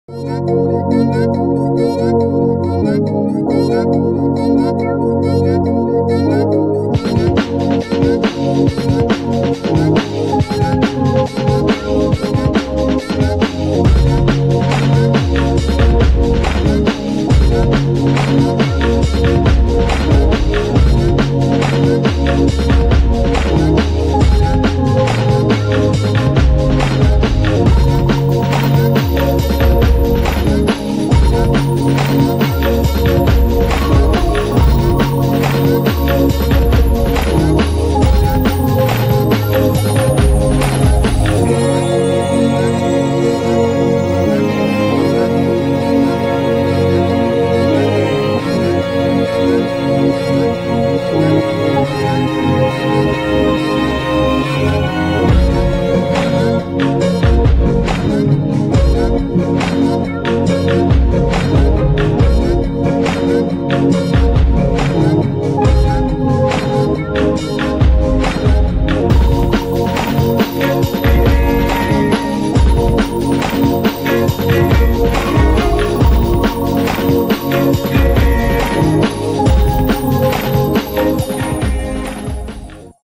Peya Jannatul | BTS 1 | Fashion Shoot | FMW
Filamint Studio - Behind The Scenes from the Fashion Shoot  of Ms. Peya Jannatul (Miss. Bangladesh 2007 & Brand Ambassador for Tresemme Bangladesh)  Location Partner:
MH - 04 The DRUNKYARD